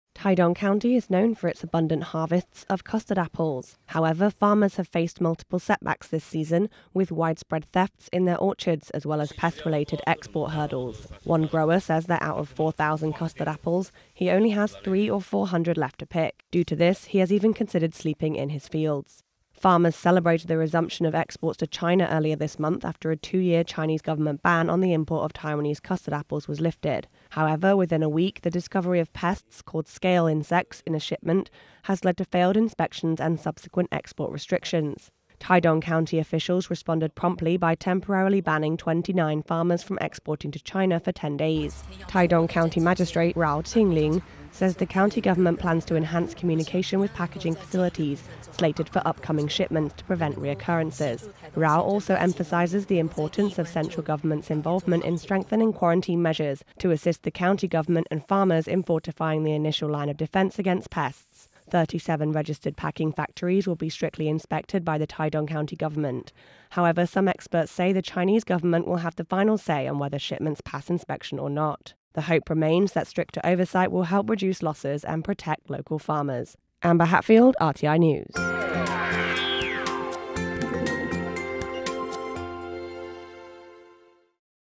Custard apple growers face multiple setbacks this harvest season | Taiwan News | RTI
Taitung County, known for abundant custard apple harvests, faces challenges this season despite the resumption of exports to China.